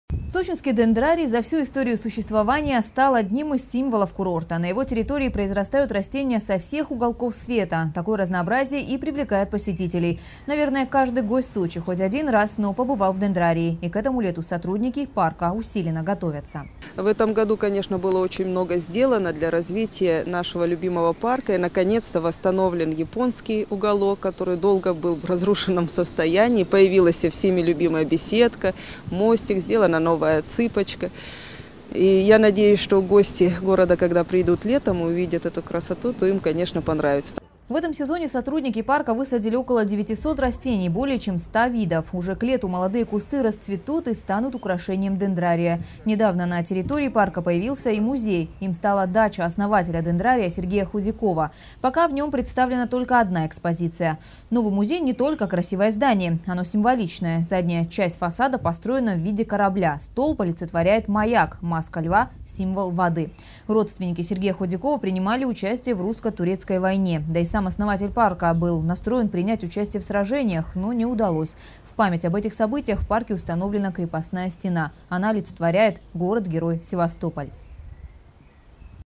Этим летом Сочи ожидает большой наплыв туристов. Новости 24 Сочи
Уверяют в этом и туроператоры. Весь Сочи готовиться принимать гостей. Не исключение и Дендрарий. Уже сегодня на территории парка обновили знаковые места и посадили более 900 новых растений.
Сочи. Новости.
Телеканал Эфкате.